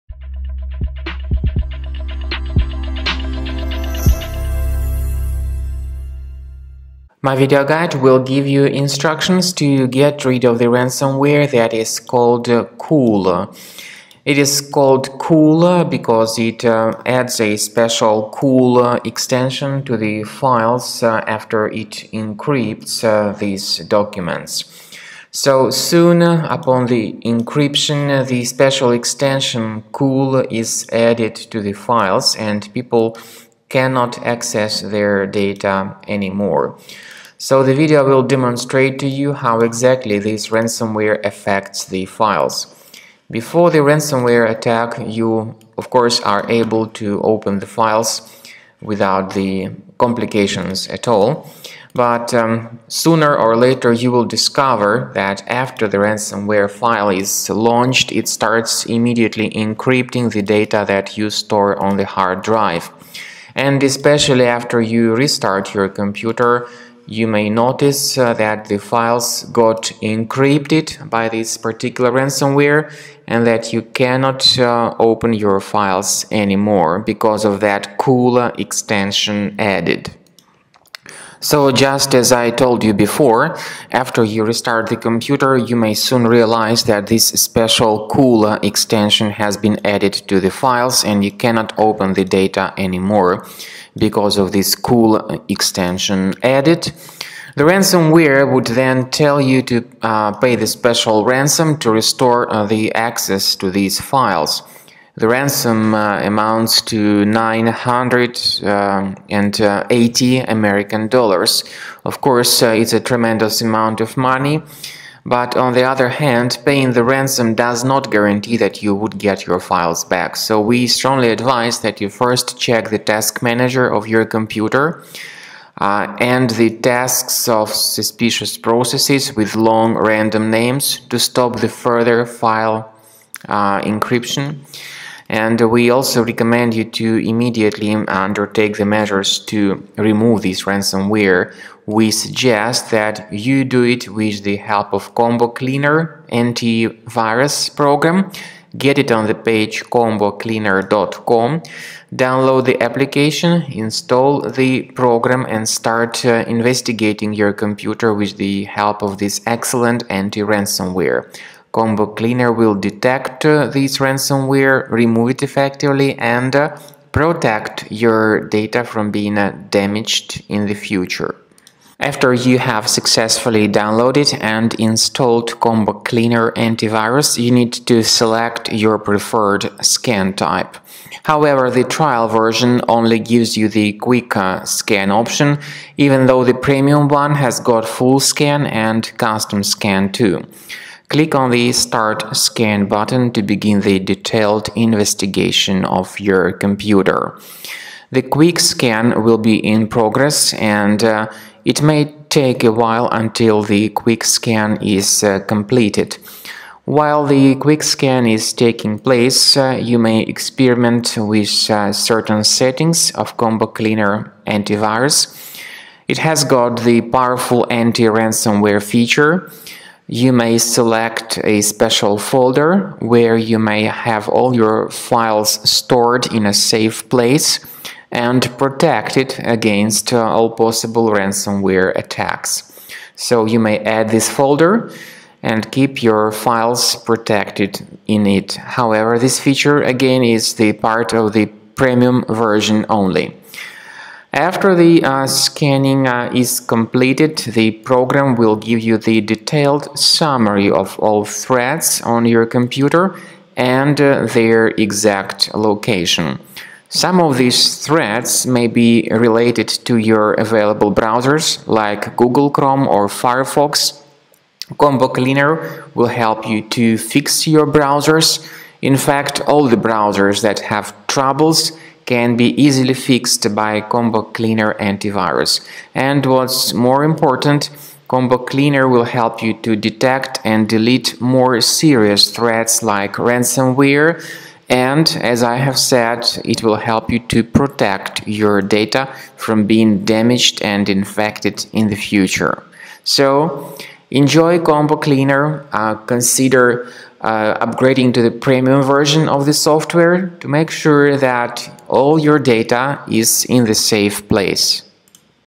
Cool Djvu file virus removal. How to restore .cool files?
Cool is a ransomware-type infection that attacks many Windows computers today. It is the part of the DJVU family of ransomware infections. It infects PCs due to certain vulnerabilities in the security system. It modifies the data and adds the special .cool extension, instructing the victims to pay money for the file restoration. Get rid of the ransomware with the help of Combo Cleaner Antivirus and get your data protected with its help in the future.